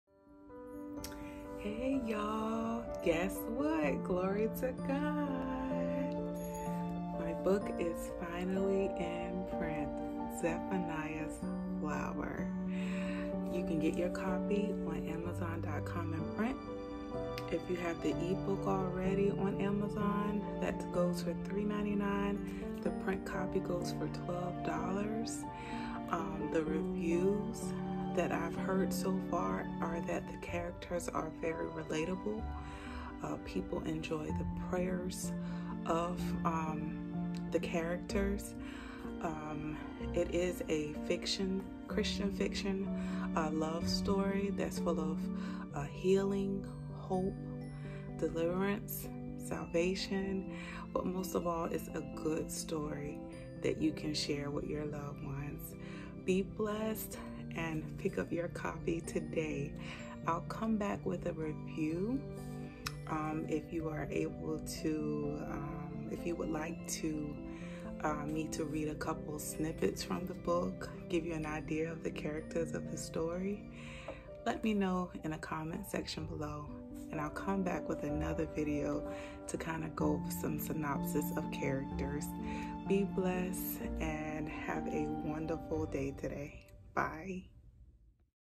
Yay! It’s here—Zephaniah’s Flower
It’s finally here! Zephaniah’s Flower is now in print. You can get your copy today on Amazon.com.

Zephaniah's Flower: A NOVELLA

If you would like me to do a live review on a chapter, please let me know in the comment section and I will come back with a live video discussing the book.

May Heaven smile upon you and give you peace.🕊